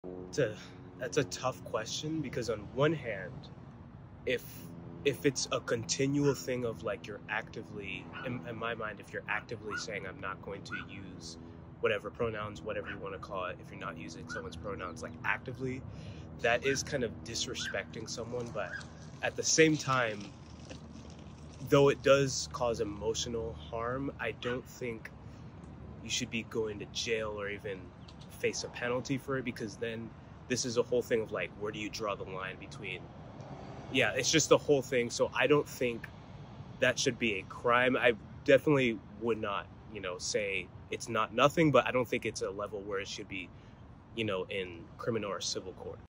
Street interview: Should misgendering be a crime?
A recent poll found 44% of millennials want to make misgendering a crime. We asked people in Seattle what they think and here’s what one man said.